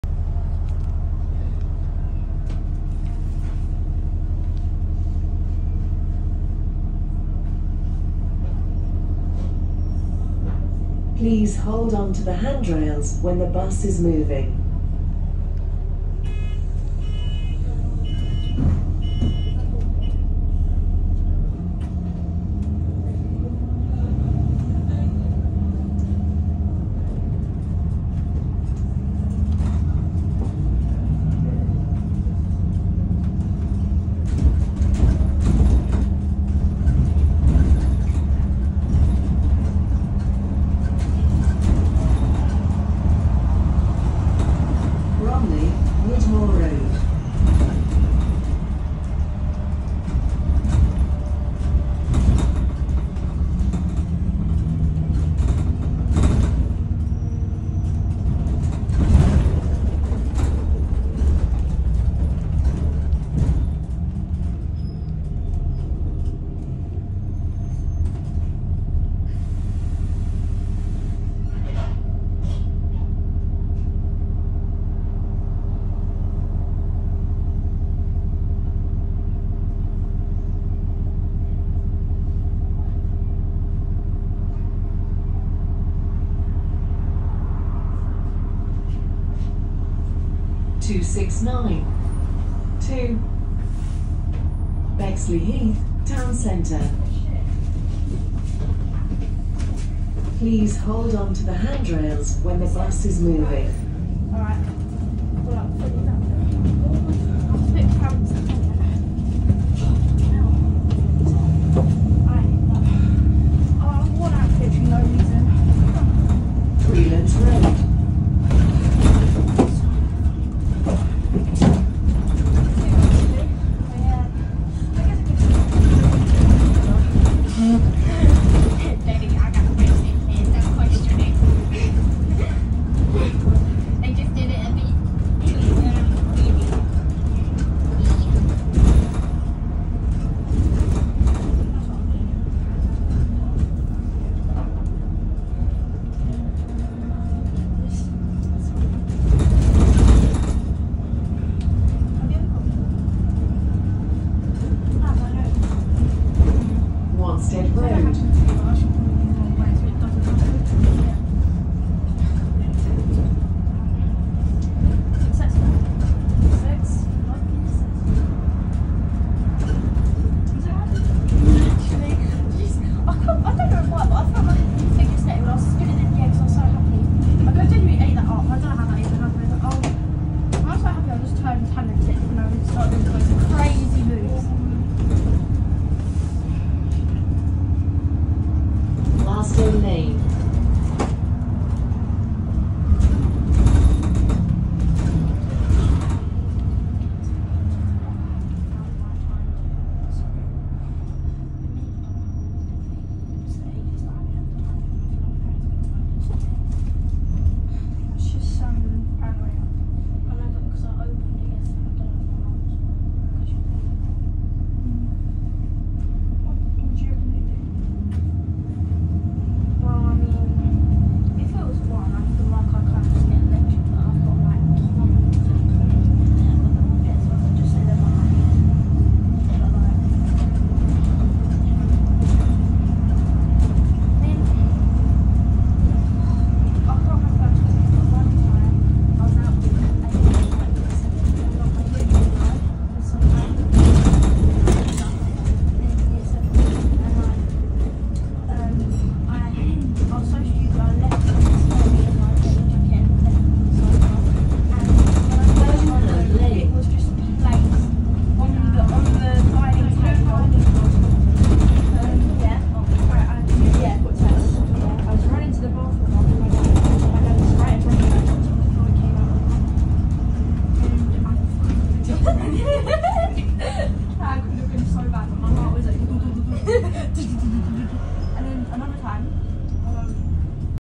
*Quiet E400*LK65 ENX Arriva London 65Reg ADL Enviro400
Journey on Arriva London’s 65Reg ADL Enviro400 On Bus Route 269 from Bromley North towards Sidcup(these 65Reg E400s are quite compared to the 59Reg E400s but let me know if you think different), Enjoy the video.